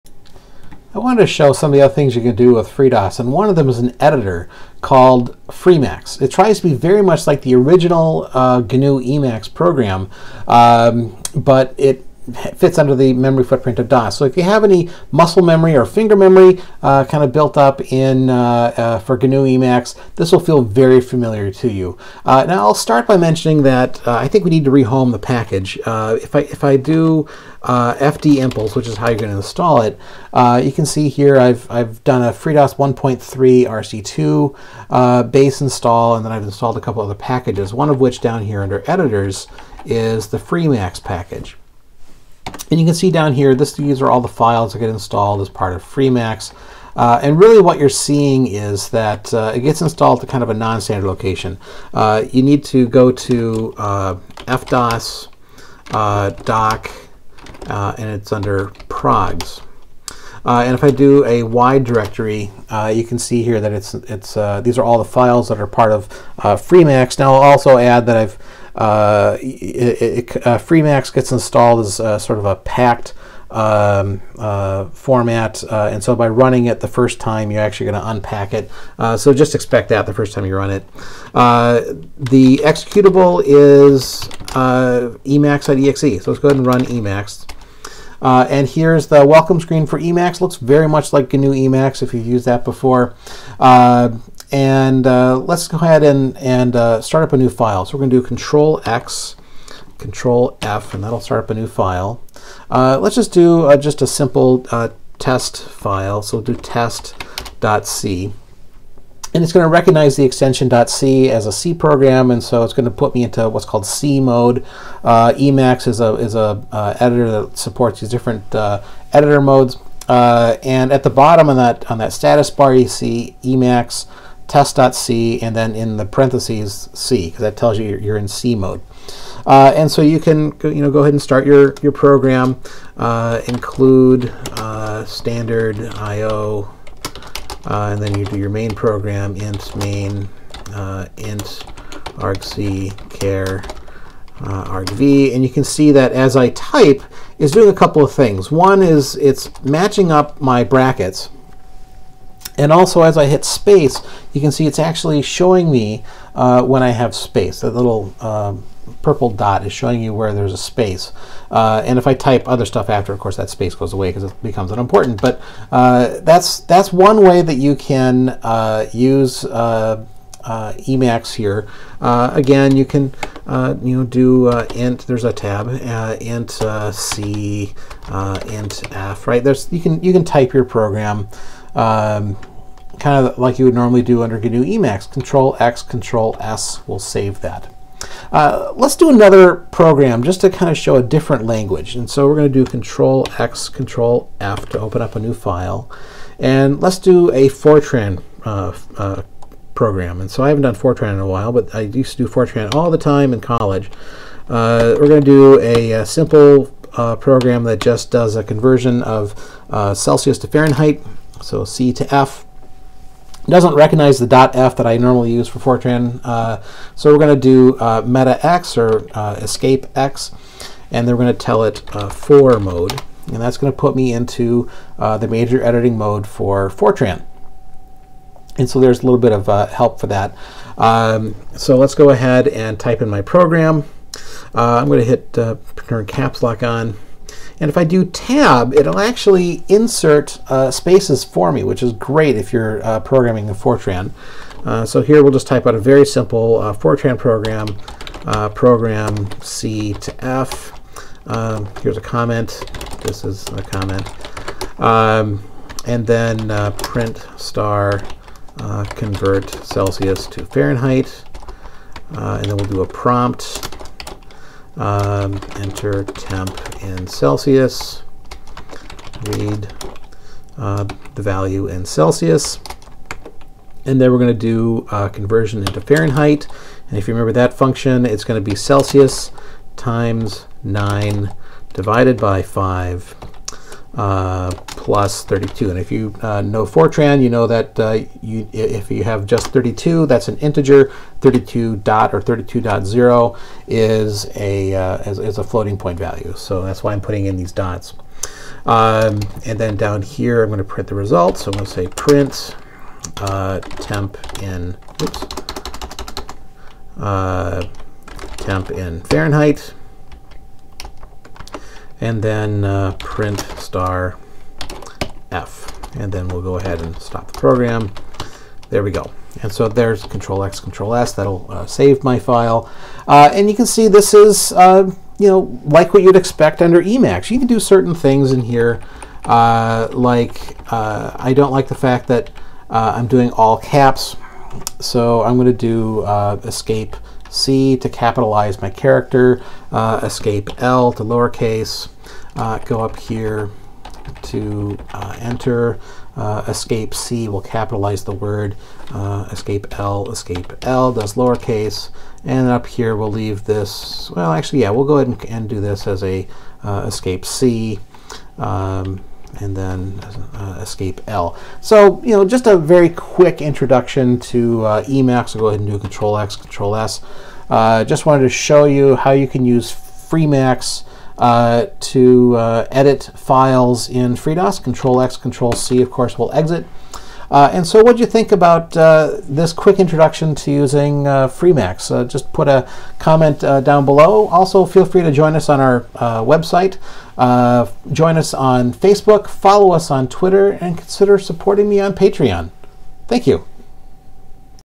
Using FreeDOS - Freemacs
If you're familiar with the GNU Emacs editor on Unix or Linux, the Freemacs editor on FreeDOS will feel very familiar to you. Freemacs tries very hard to be similar to GNU Emacs.